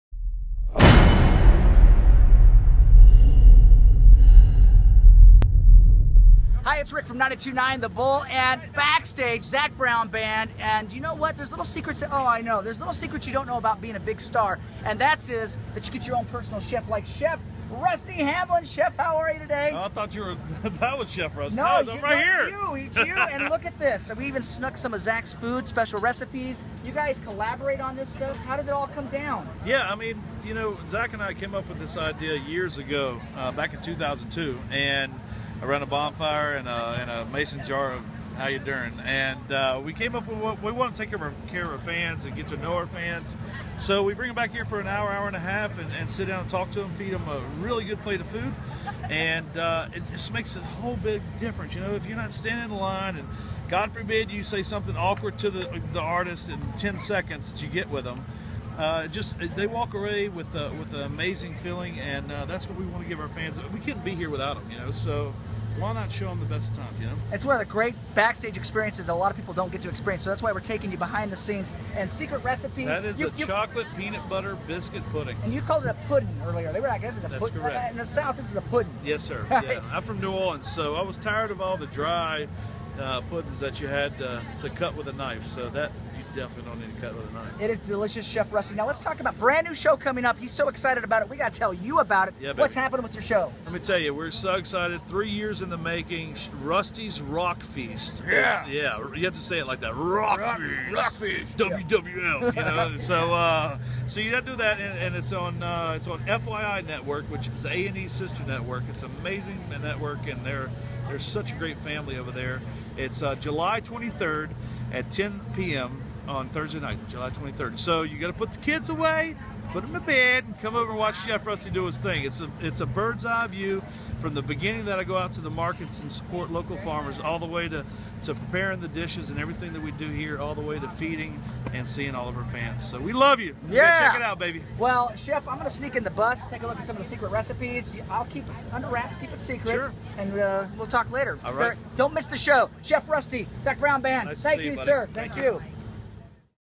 Chef Rusty Talks The Eat and Greet and his new show on the FYI Network
Rik Mikals visits with Chef Rusty Hamlin about the exclusive Zac Brown Band Eat and Greet and his brand new TV show on the FYI Network, "Rusty's Rock Feast."